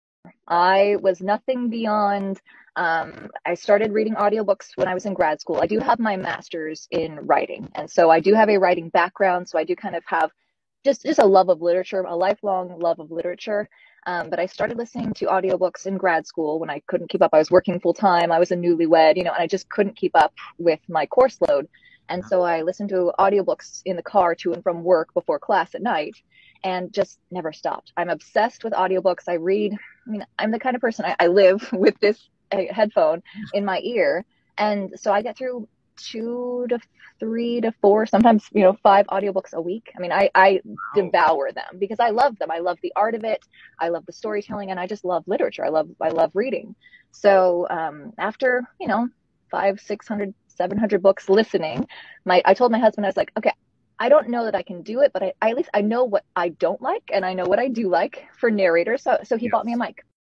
Getting Started As An Audiobook Narrator
I had no idea book narration involved so much! Thank you, Laura Horowitz, for giving us a behind-the-scenes look at what you do.